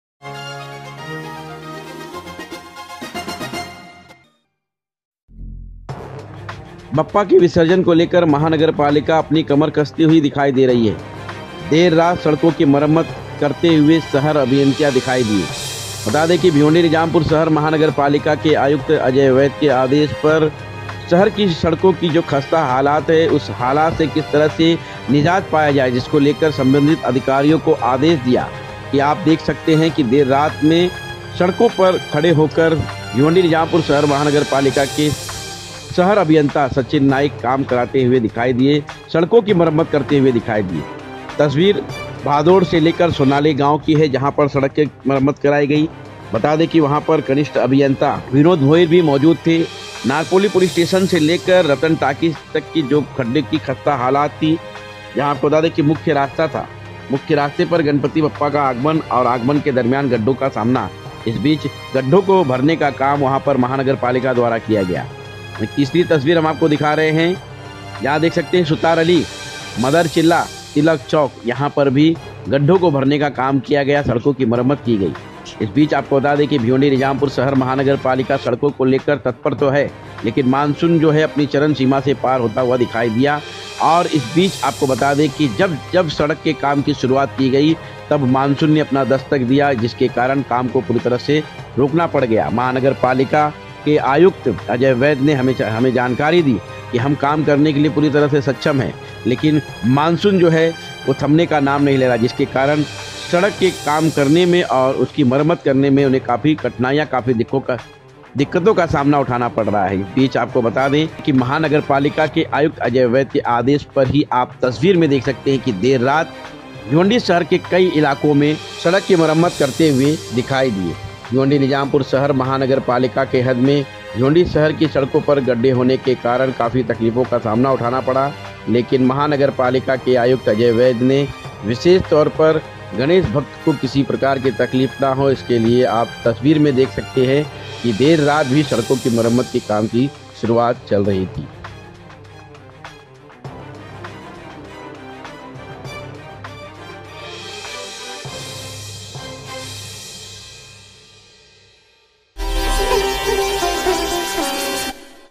bhiwandi देर रात तक चलता रहा काम| Ganpati visarjan ko lekar mahanagarpalika ne Kashi apni kamar#top
भिवंडी निजामपुर शहर महानगरपालिका के आयुक्त अजय वैद्य के आदेश पर भिवंडी शहर की सड़कों की मरम्मत देर रात तक की गई बता दे की भिवंडी निजामपुर शहर महानगरपालिका की आयुक्त अजय वैद्य इन दिनों काफी चिंतित थे क्योंकि मानसून थमने का नाम नहींले रहा जब सड़क के काम की शुरुआत की जाती है तब मानसून अपना दस्तक देता है जिसके कारण सड़क की खस्ता हालत जैसीपड़े रहते हैं सवही महानगर पालिका के आयुक्त अजय वैद्य बरसात रुकने का समय देख रहे थे कि समय रुक की काम शुरू हो गया महानगर पालिका के कई संबंधित अधिकारी सड़कों पर उतर कर देर रात तक की सड़क की मरम्मत करते हुए दिखाई दिए..!